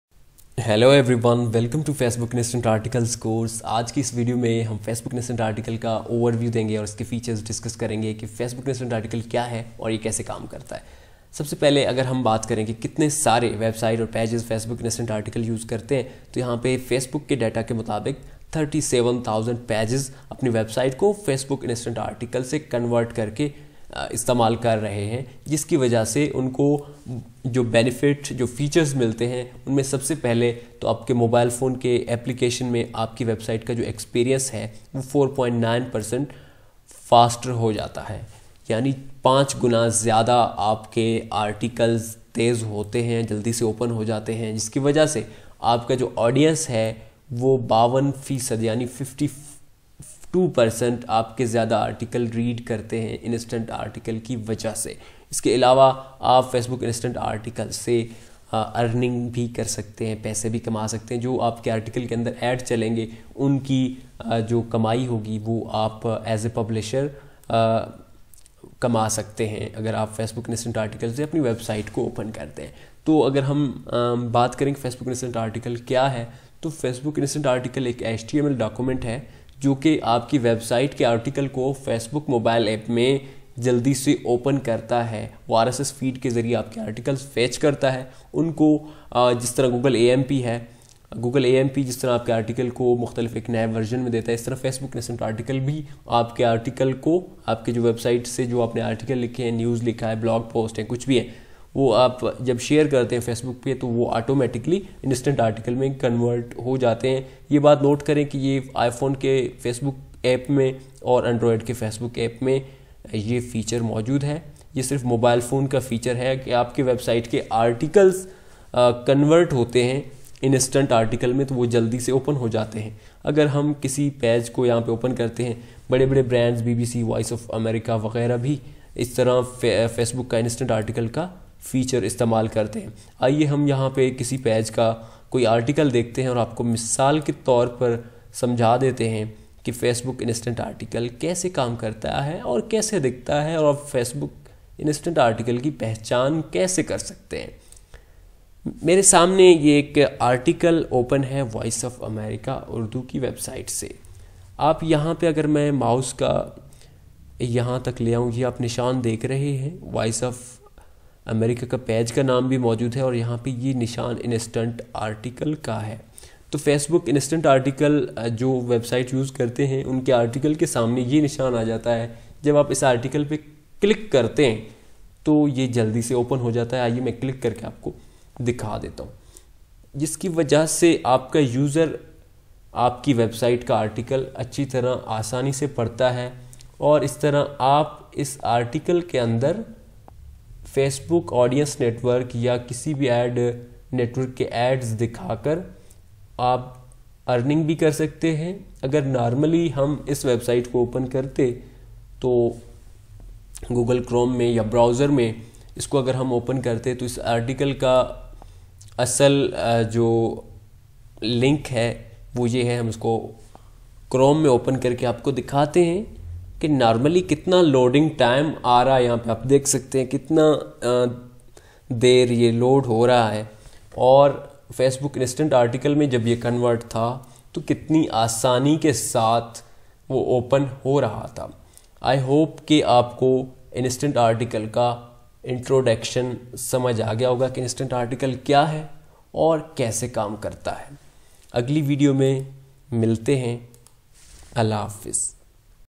Facebook Instant Articles with NPI Advanced Course | Platform Overview
In this Video you will learn What is Facebook Instant Articles and How It works. This is First Video of IA advanced course Series. In This Course we will learn How to approve Instant Articles in 2022 with News Page Index.